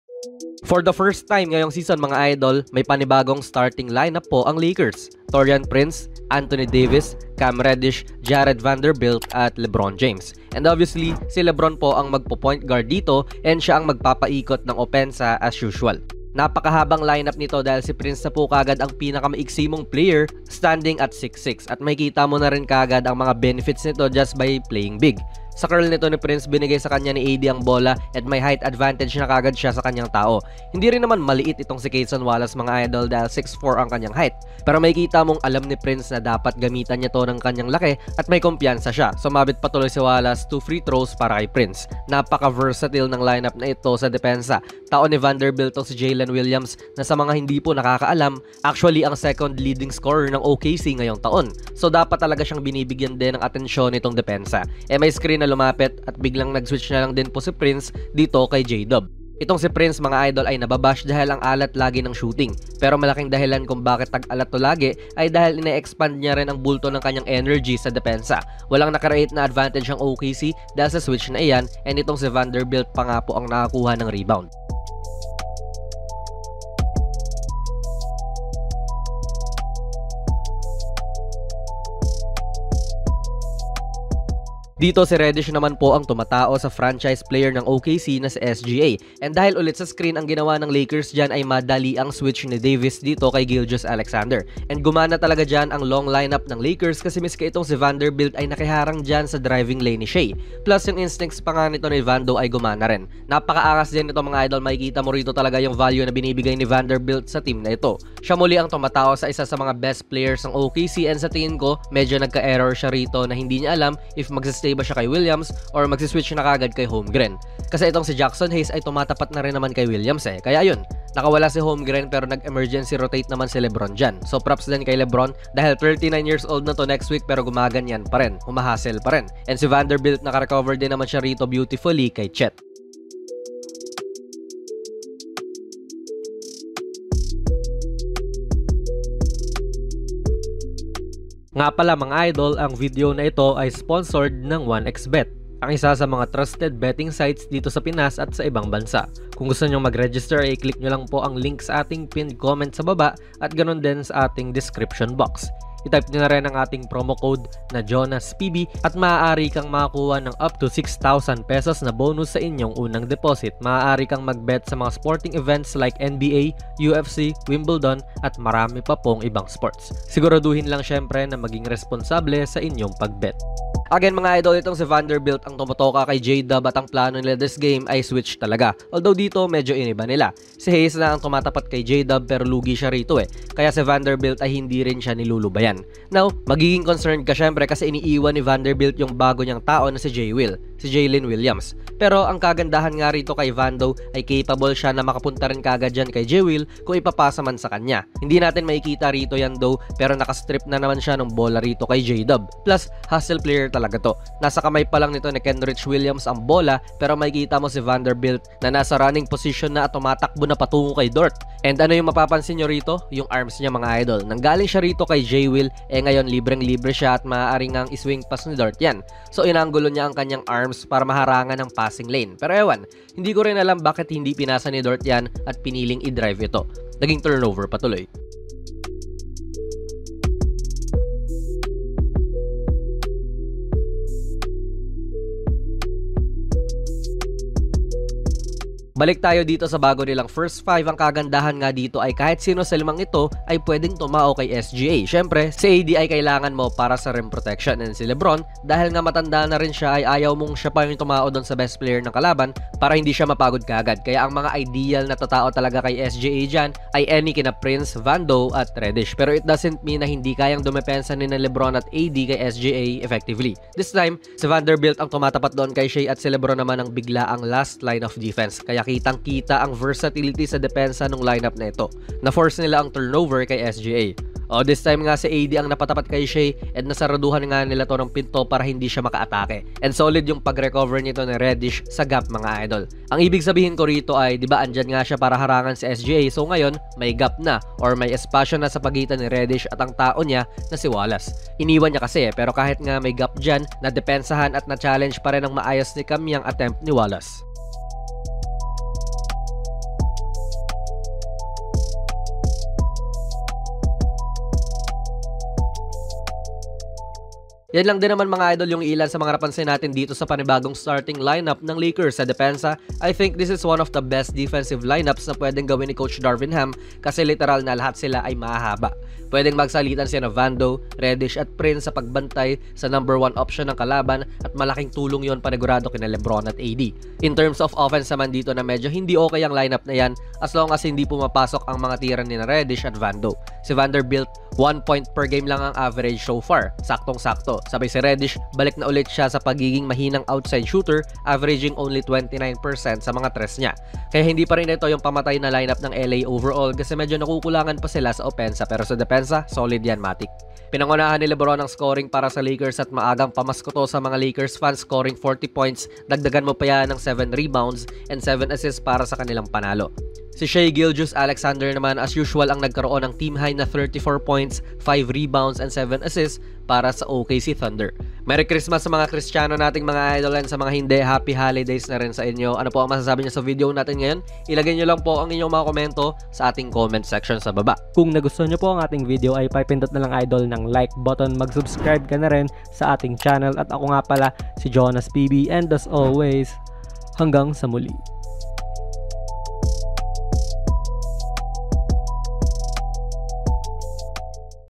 Ganito ang ginawa ng MAHABANG STARTING LINEUP ng Lakers kontra OKC
Bagong starting 5 ng Lakers, kumusta?